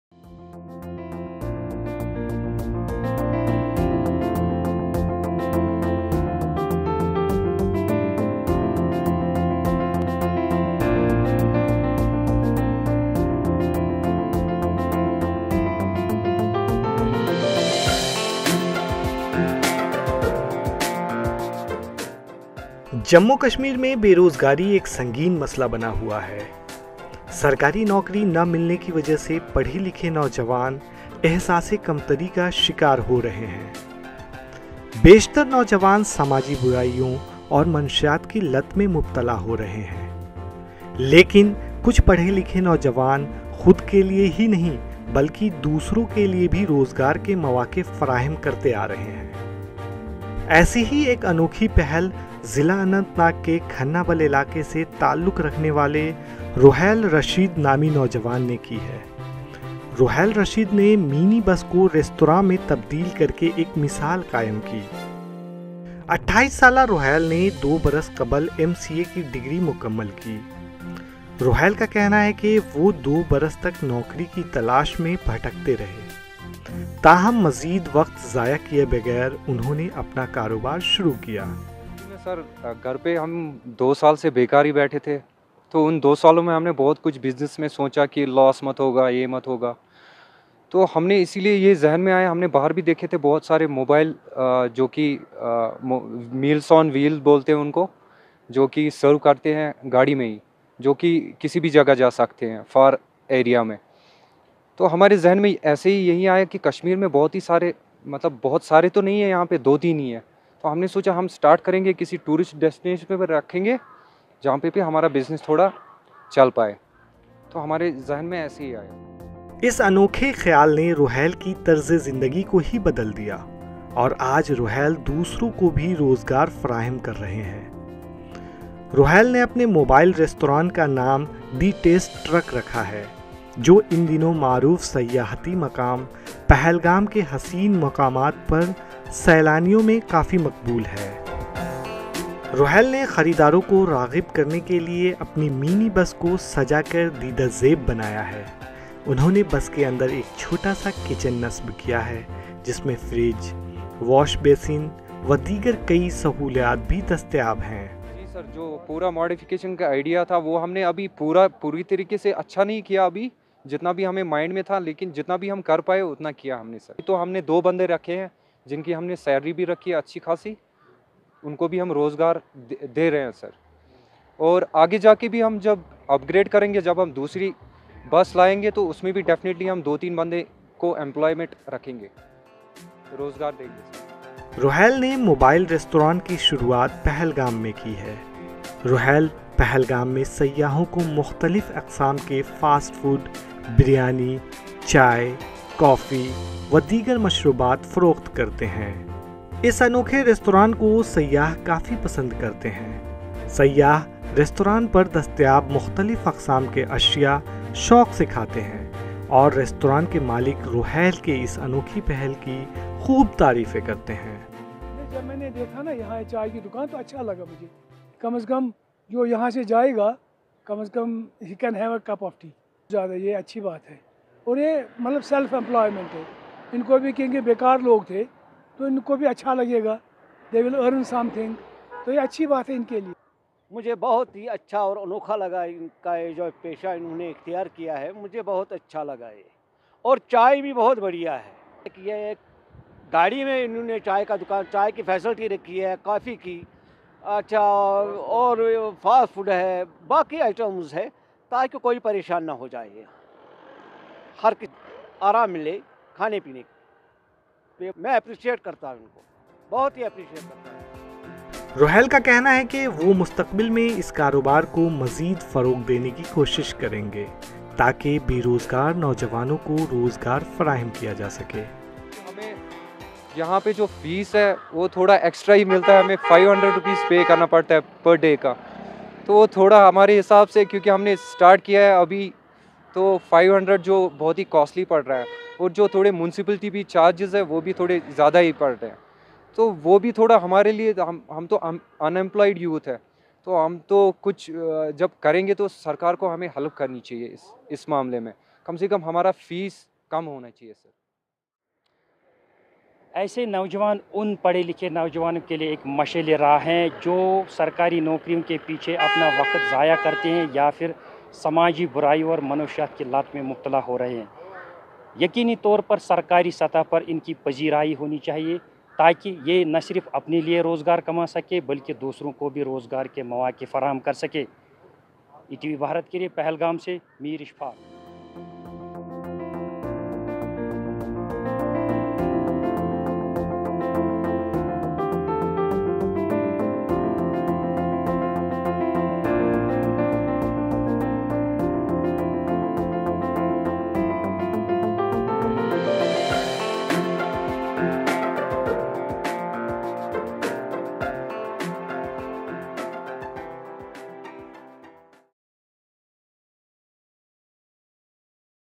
Restaurant on Wheels In Pahalgam | اننت ناگ کے تعلیم یافتہ نوجوان کی انوکھی پہل | Unique Restaurant
اننت ناگ: جموں و کشمیر میں بے روزگاری ایک سنگین مسئلہ بنا ہوا ہے۔سرکاری نوکری نہ ملنے کی وجہ سے پڑھے لکھے نوجوان احساس کمتری کا شکار ہوتے ہیں، بیشتر نوجوان سماجی برائیوں اور منشیات کی لت میں مبتلا ہو رہے ہیں، لیکن کچھ پڑھے لکھے نوجوان خود کے لیے ہی نہیں بلکہ دوسروں کے لیے روزگار کے مواقع فرام کرتے ہیں۔Unemployment In Kashmir